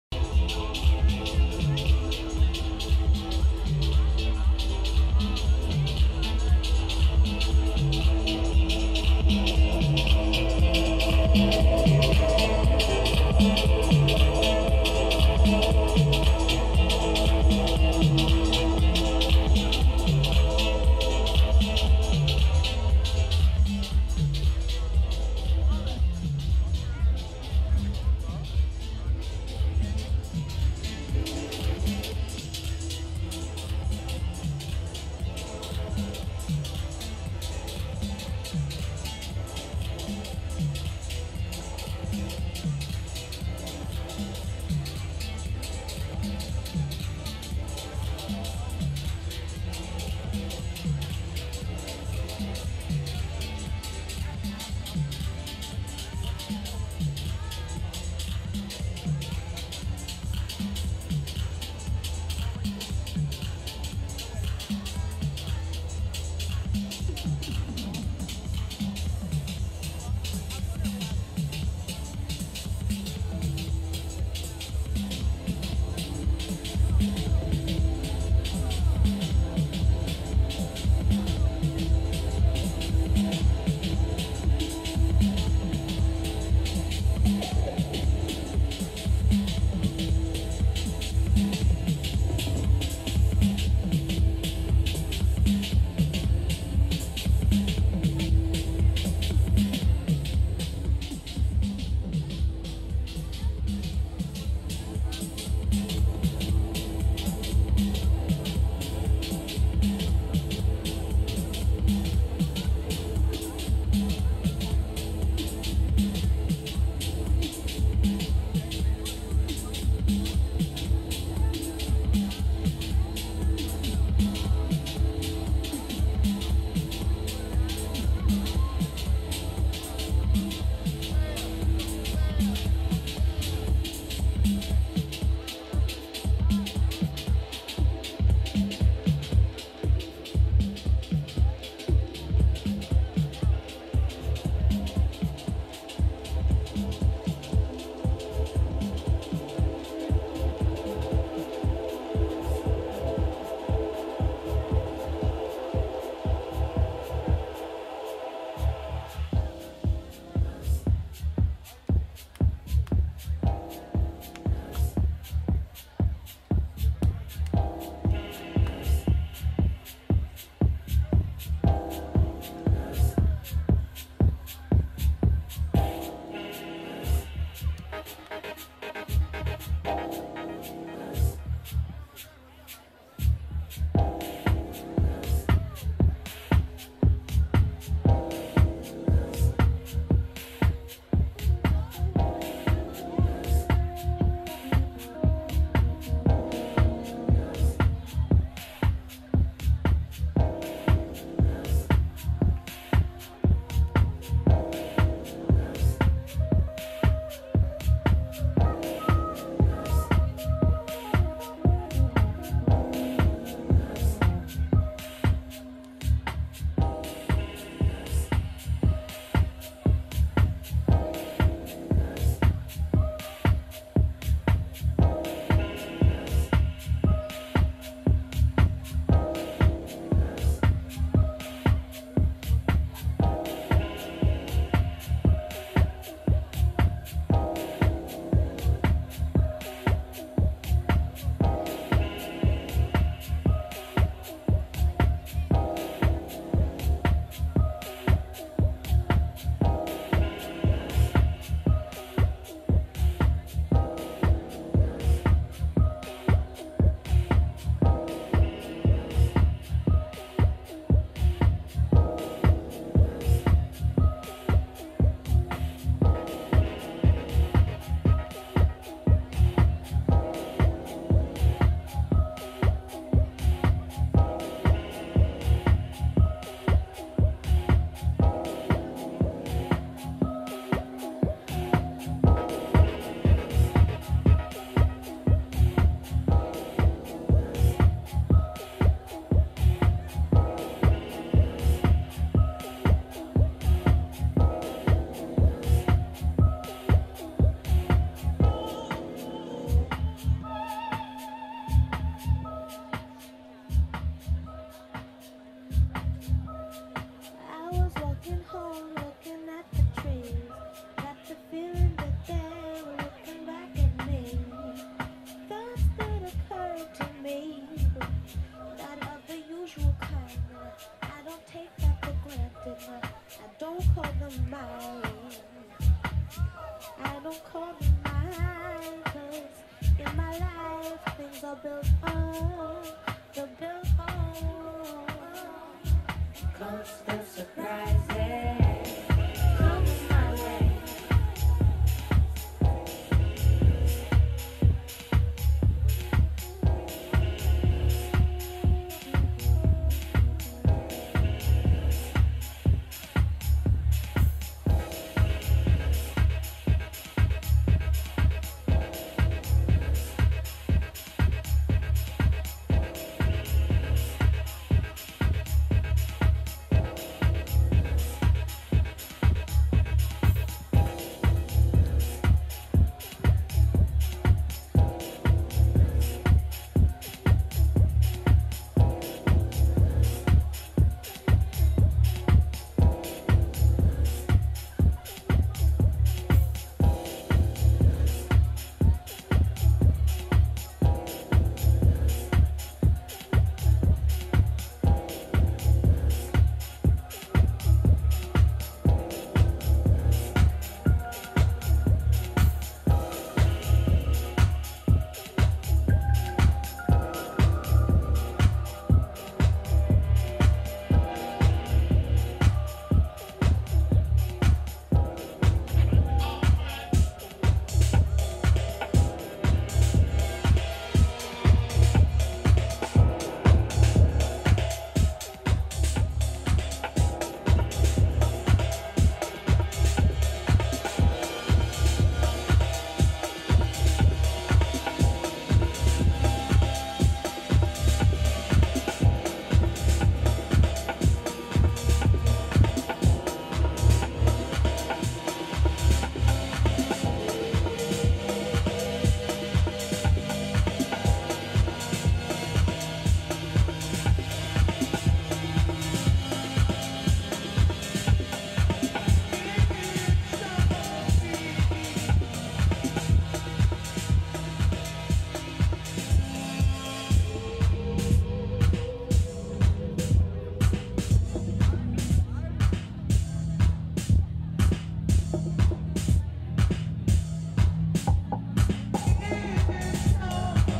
Mister Sunday: Eamon Harkin & Justin Carter II (May 29 2011)
Eamon Harkin & Justin Carter open for Four Tet at Mister Sunday, Gowanus Grove Brooklyn, May 29 2011